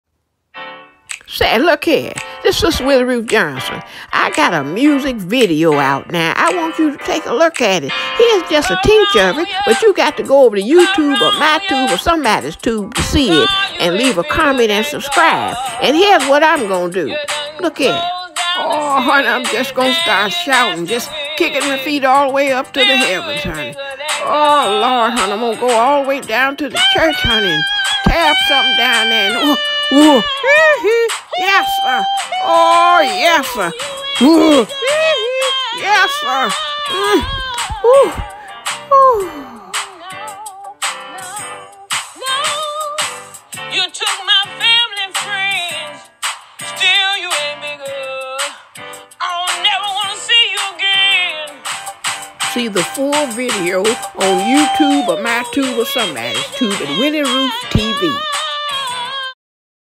Comedienne Willie Ruth Johnson promotes new music
America’s beloved big mama, Sistah Willie Ruth Johnson has a new single and a music video! C’mon go take a look —this is just a trailer. (Full video)